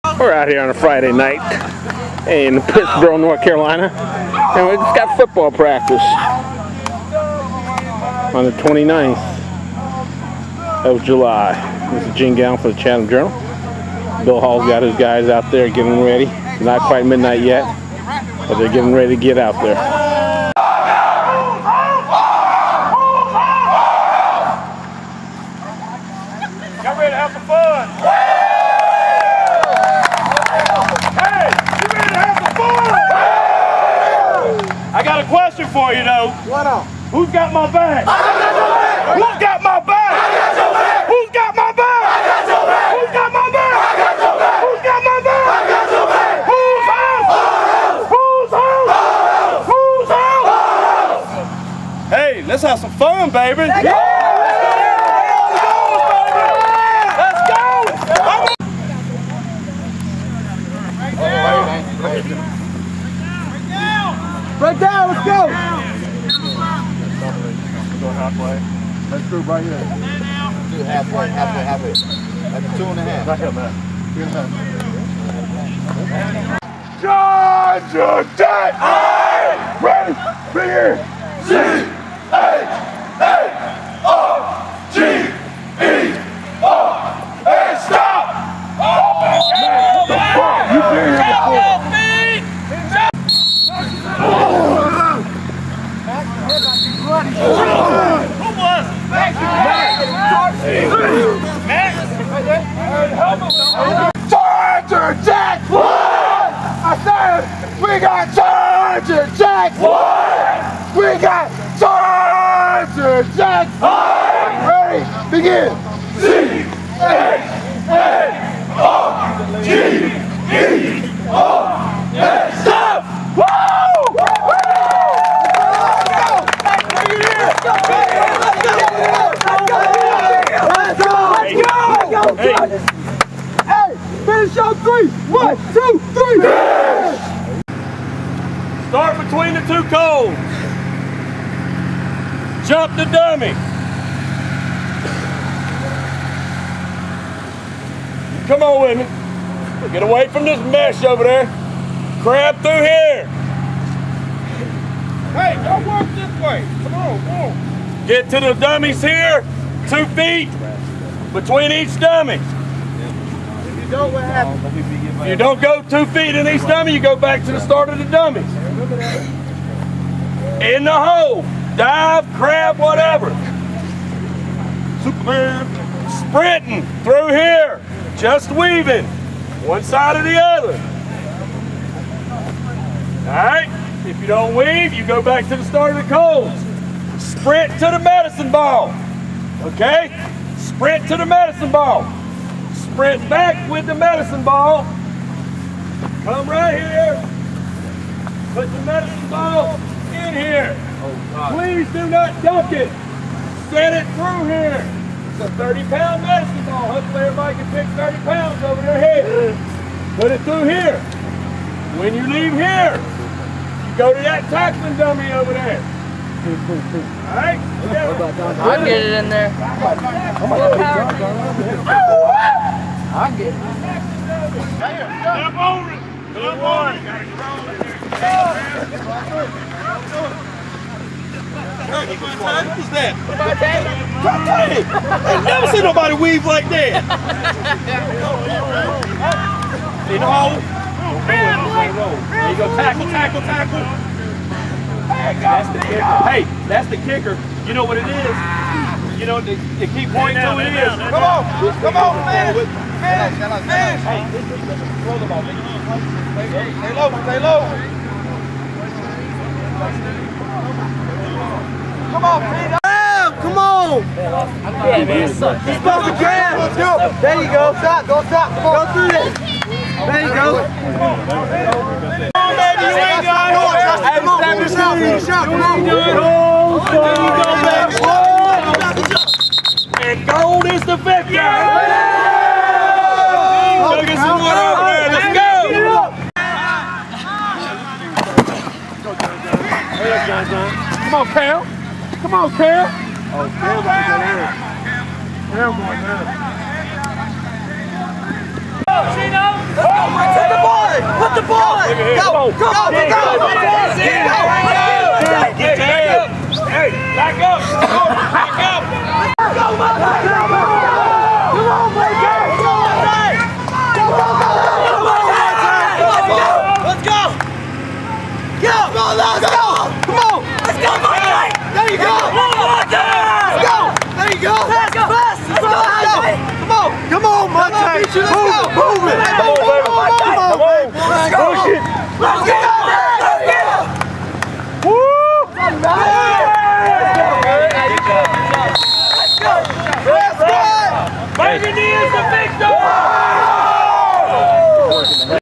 2011 Northwood Chargers High School Midnight Football Practice
Coach Bill Hall kicks off the 2001 Northwood Chargers High School Midnight Football Practice in Pittsboro, NC. Filmed by Gene Galin for the Chatham Journal newspaper.